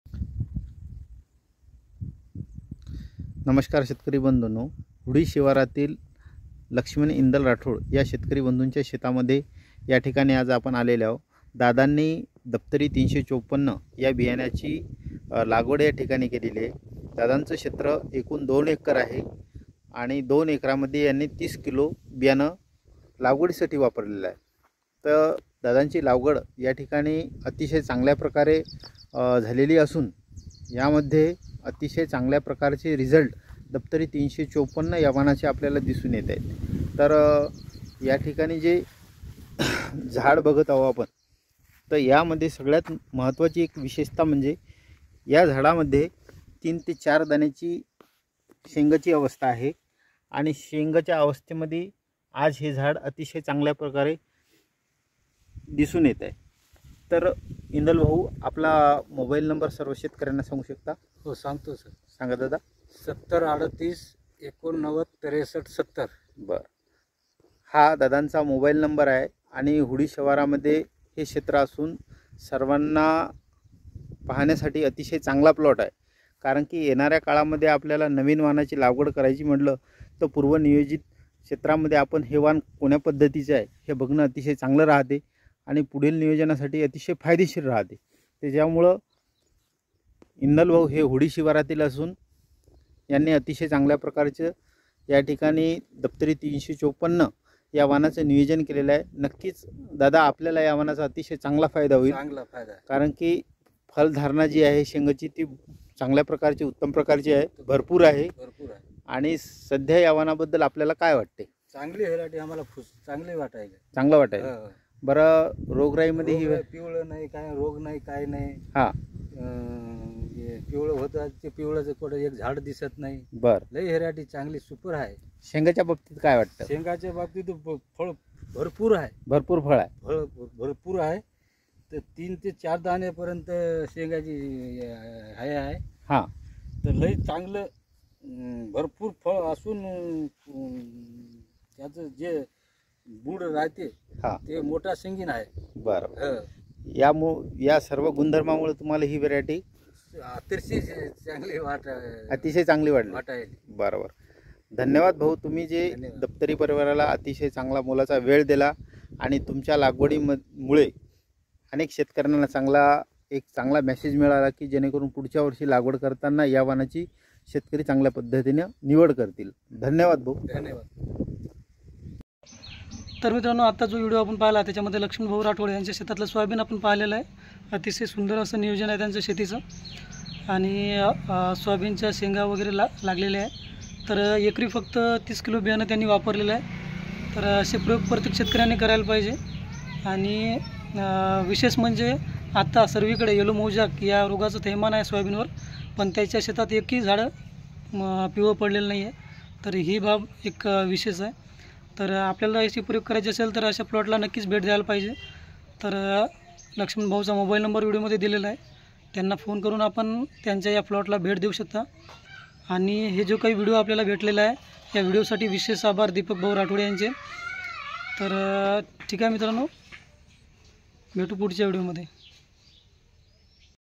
सोयाबीन शेतीचे नियोजन एकदा बघाच
स्वतः तालुक्यातील होळी या गावचे प्रयोगशील शेतकरी लक्ष्मण भाऊ राठोड यांच्या शेतामधील सोयाबीनचा व्हिडिओ आपण पाहत आहोत अतिशय सुंदर नियोजन पद्धतीने सोयाबीनची लागवड या ठिकाणी झालेली आहे आपल्याला हा प्लॉट नक्कीच आवडेल